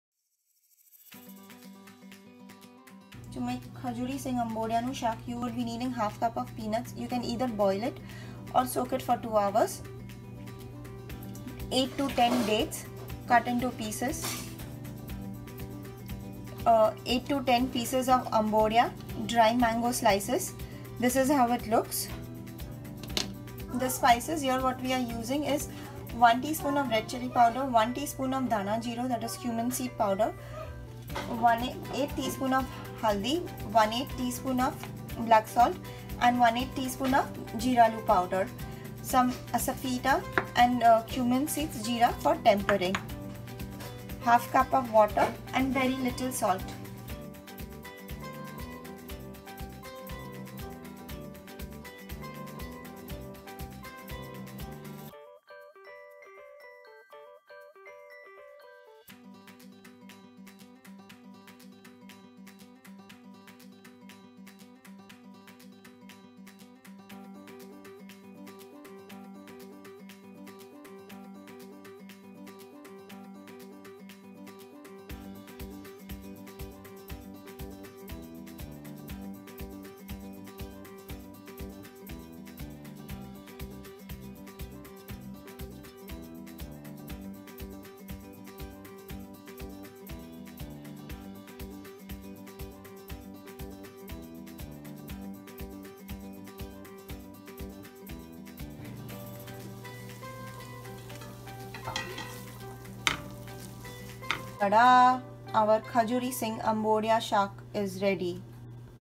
Khajori Sing Ambodia nu Shak - Paryushan Tithi Special
1/2 cup of Peanuts (sing) (soaked for 2 hrs and boiled), 8-10 pcs of Dates, 8-10 pcs of Ambodia (dry mango slice/ aamchur slice).
Spices: 1 tsp of each: red chili powder, dhanajeera powder
1/8 tsp of each:  haldi, blacksalt, jeeralupowder, asafetida, jeera and little salt.
1/2 cup of water.